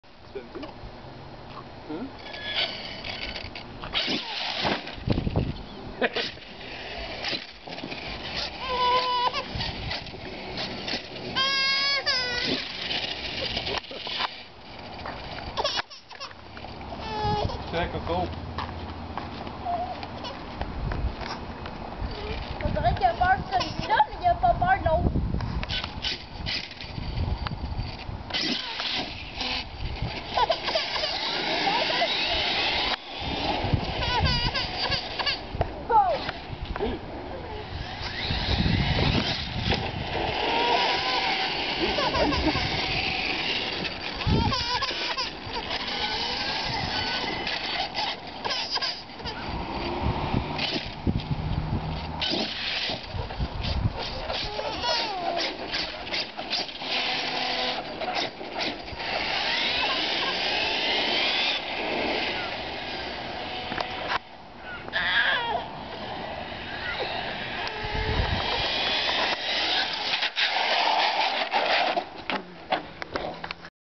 Hpi savage flux hp maiden run, crazy!!!!!
This is my new savage flux hp. I got this 2 days ago for my birthday, i'm running it with 3 s lipo 5000mah
this is crazy shit compare to my savage XL, it as twice the power as the k5.9 and ton of torque